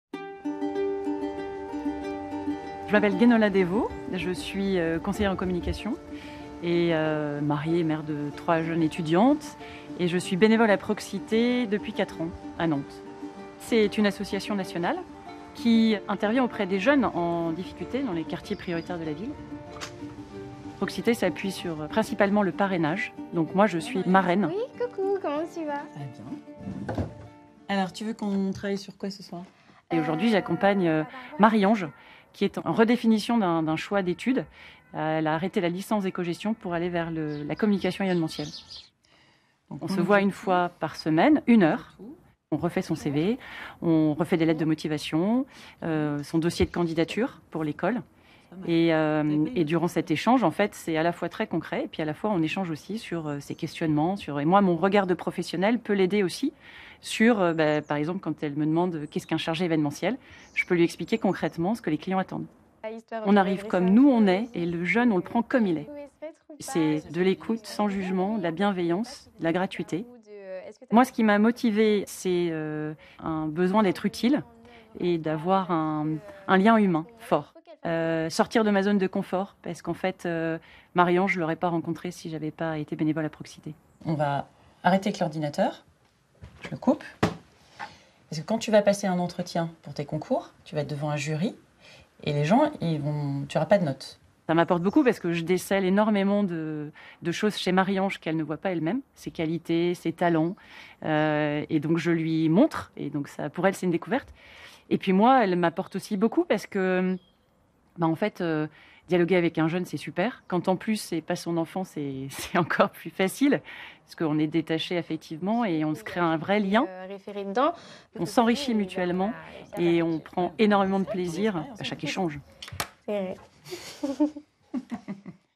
Guénola Désveaux, bénévole à l’association Proxité | Vous nous étonnez
Rencontre avec Guénola Désveaux, bénévole à l'@associationproxite2347.